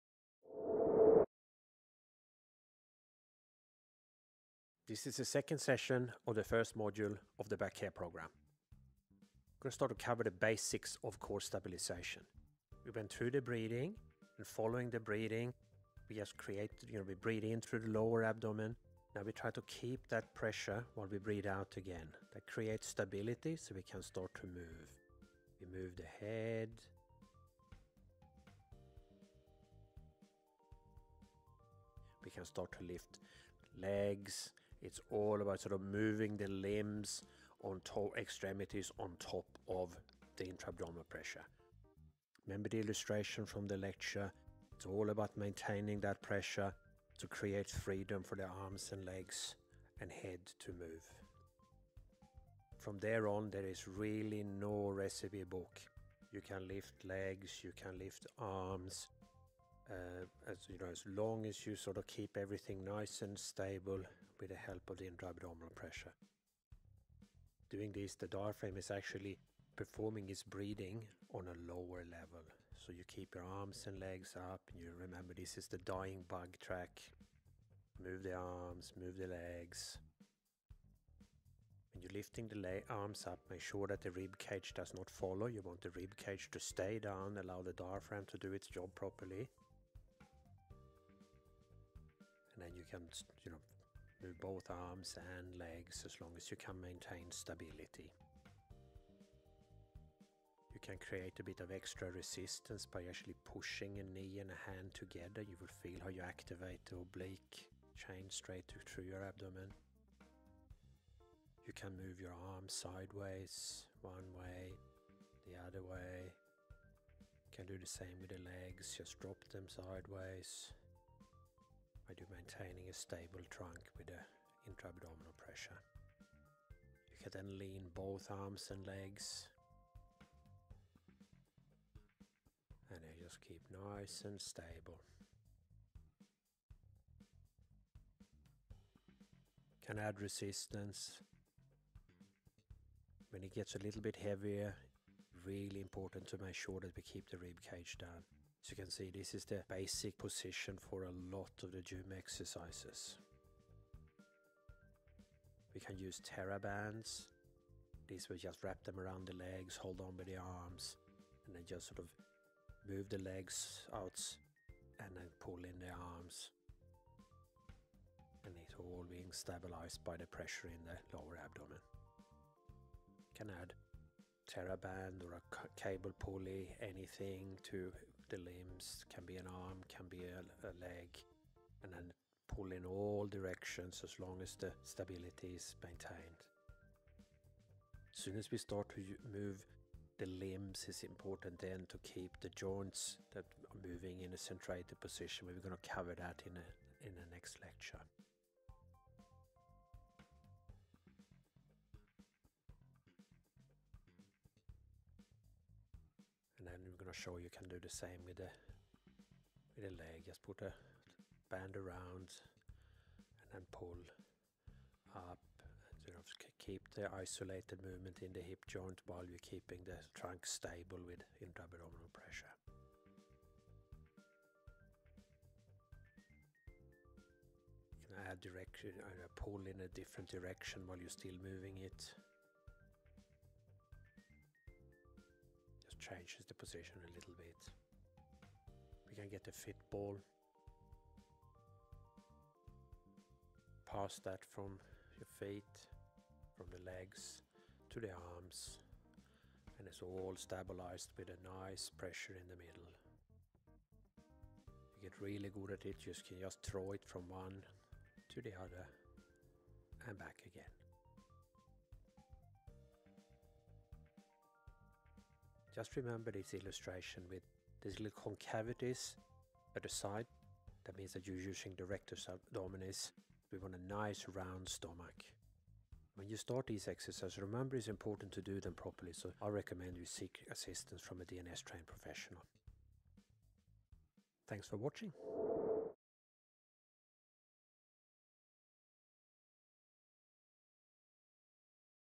Core Stabilisation M1S2 | Back Care Program
The second session in Module 1 of the Back Care Program.
Following on from the first session, in this video we look at some of the basic core stabilisation exercises.